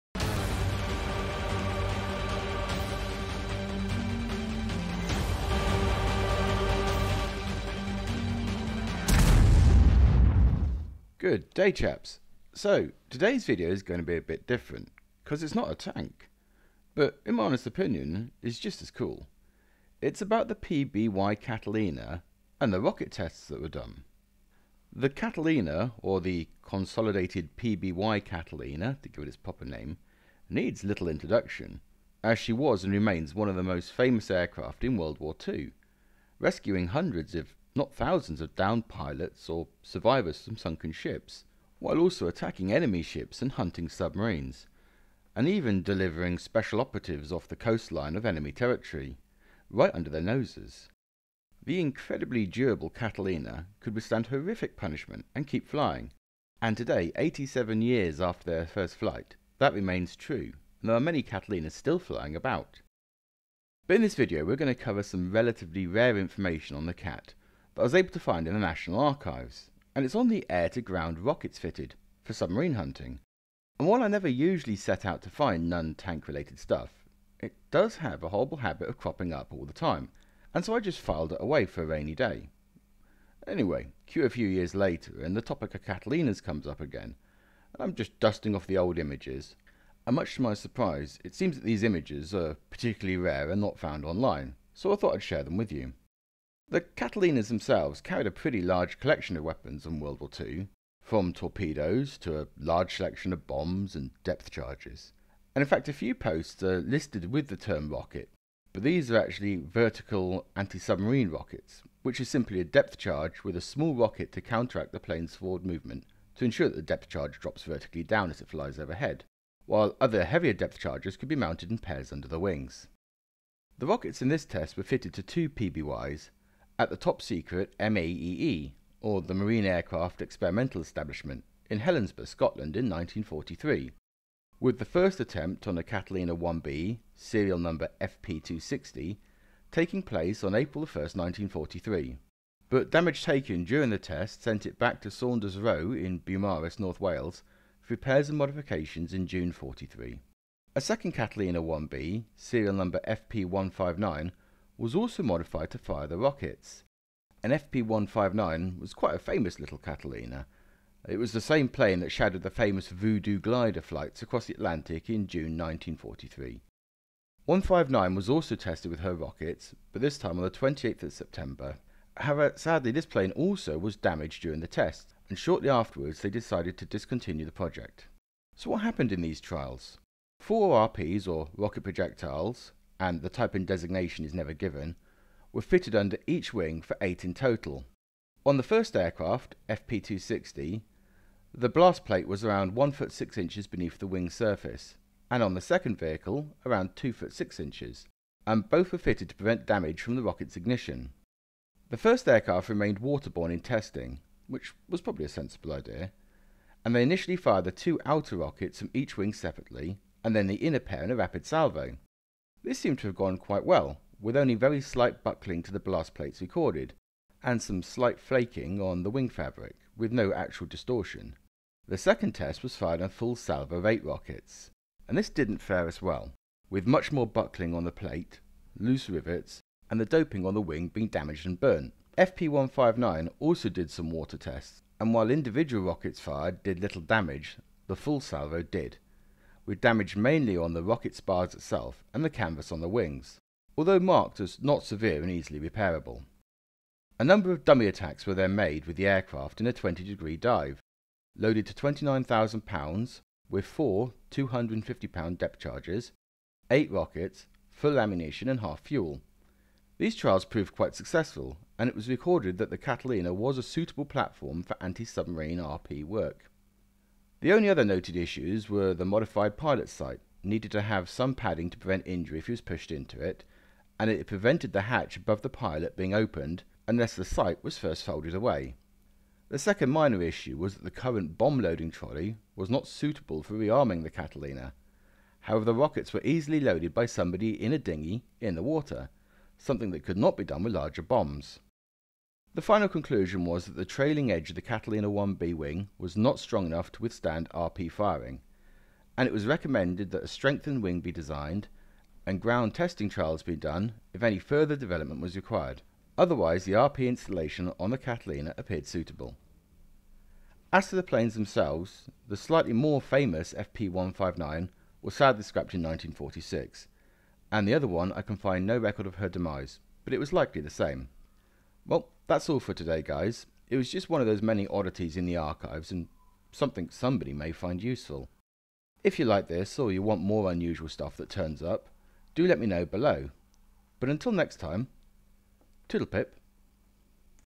PBY Catalina Rocket Tests
Today’s video is a bit different, its not a tank, but a flying boat, the famous Catalina and the Rocket tests done to see if the plane could mount and fire rockets at submarines. These tests re very little know about and these are (as far as I know) the first pictures online of the rocket tests and will fill in some gaps not only on the Catalina but on the individual aircraft as well.